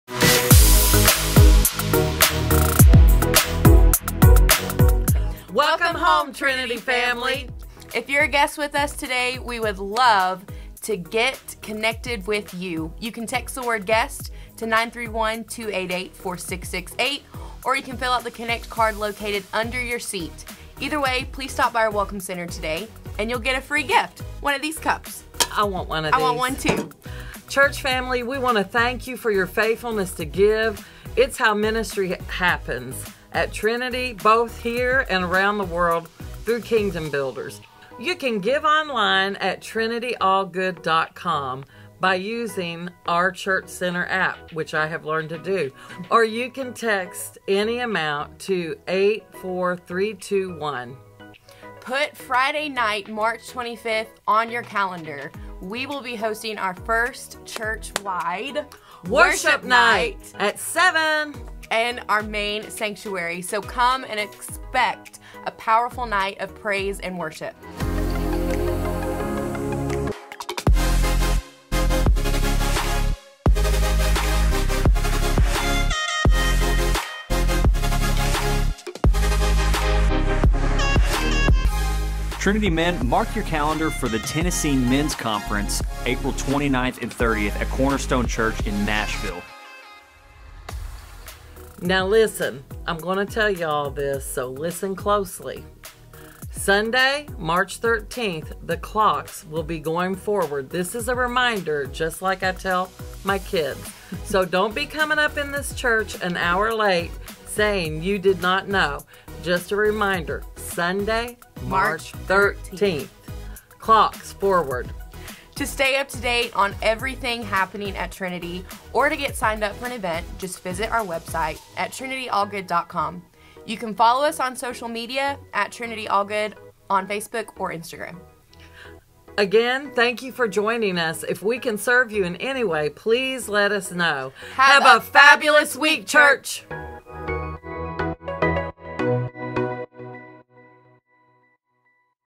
Trinity news for the week of March 6
Worship Night, Tennessee Men's Conference, Daylight Savings